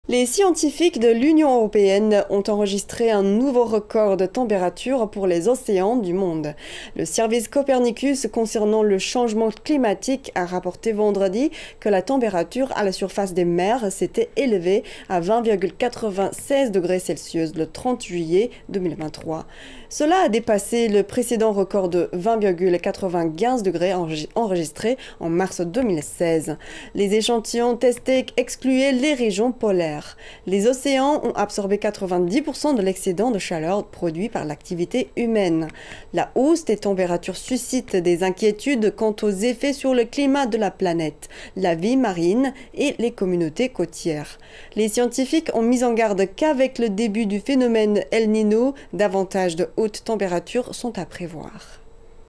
La température de surface des mers atteint le record de 20,96 degrés Celsius
Les scientifiques de l'Union européenne ont enregistré un nouveau record de température pour les océans du monde. Le Service Copernicus relatif au changement climatique a rapporté le 4 août que la température à la surface des mers s'était élevée à 20,96 degrés celsius le 30 juillet 2023. Ce chiffre a dépassé le précédent record de 20,95 degrés enregistré en mars 2016. Les échantillons testés excluaient les régions polaires. Les océans ont absorbé 90% de l'excédent de chaleur produit par l'activité humaine. La hausse des températures suscite des inquiétudes quant aux effets sur le climat de la planète, la vie marine et les communautés côtières. Les scientifiques ont mis en garde qu'avec le début du phénomène el Nino, davantage de hautes températures sont à prévoir.